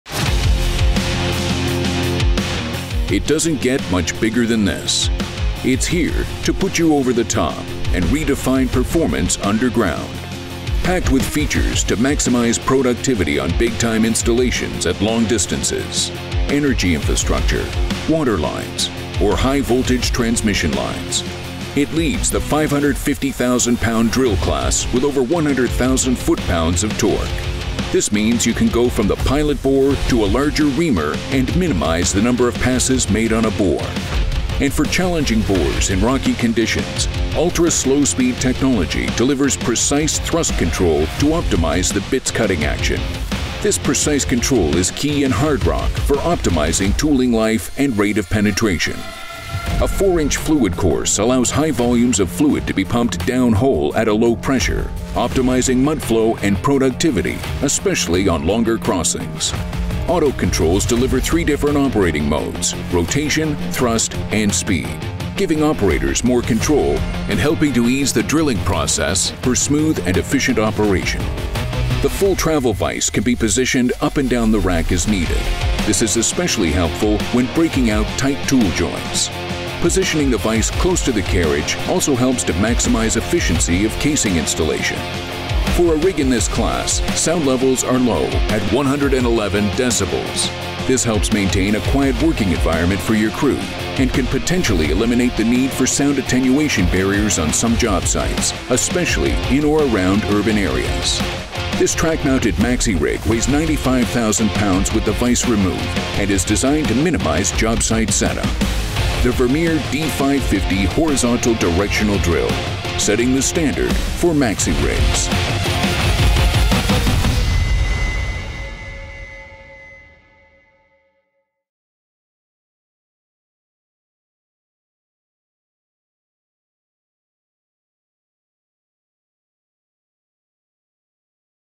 Features and benefits of the D550 horizontal directional drill
The D550 horizontal directional drill (HDD) is packed with features to maximize productivity on big-time installations at long distances — energy infrastructure, water lines or high-voltage transmission lines. Learn more about this class-leading directional drill and how it's setting the standard for maxi rigs.